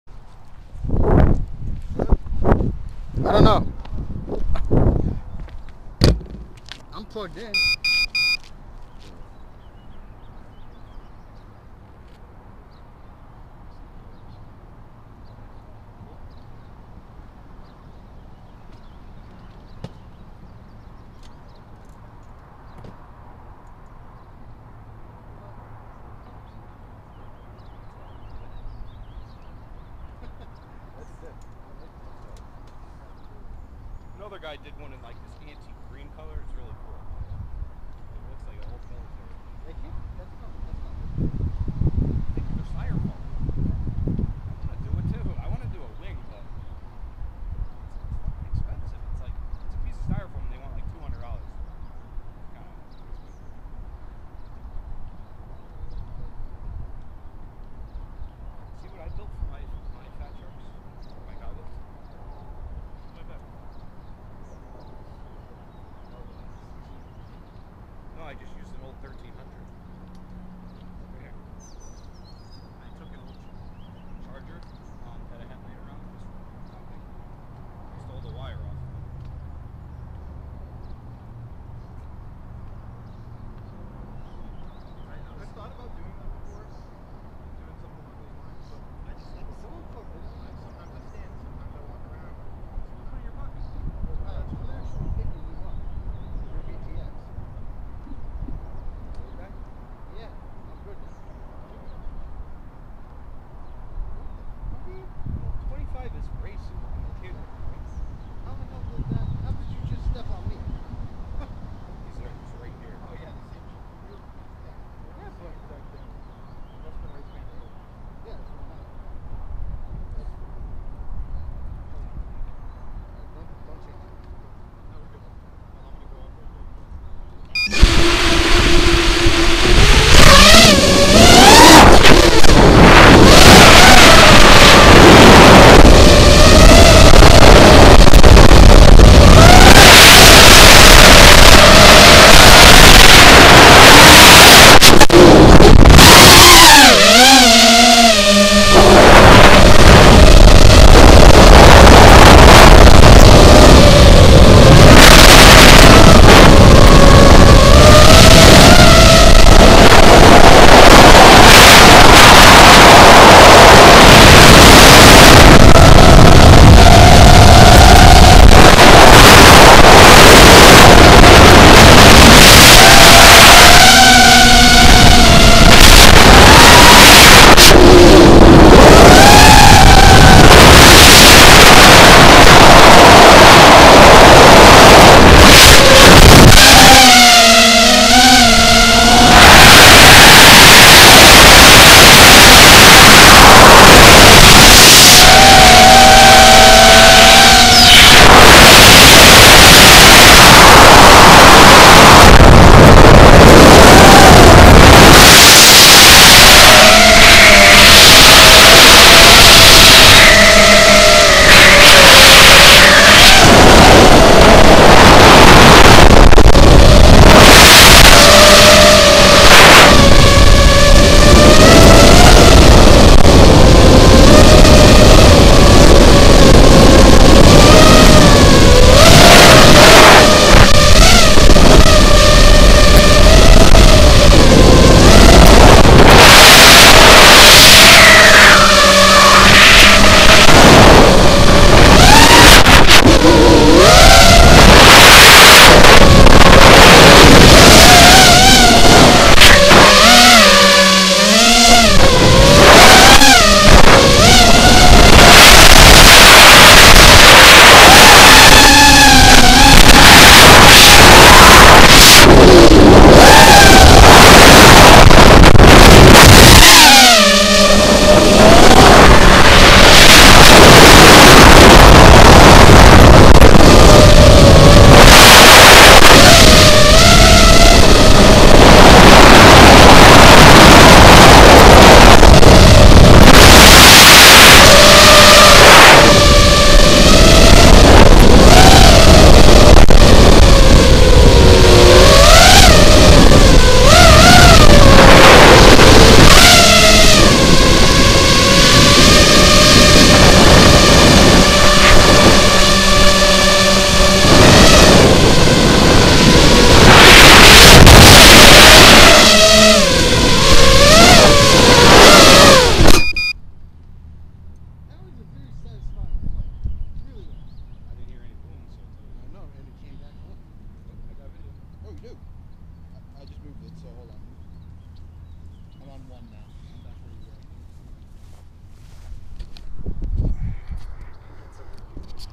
PSEKAS - CTFPVRACING - Orange Test Flight - New Location Scout Mission
testing all signals and the area, also improving my obstacle avoidance skills, little by little! :)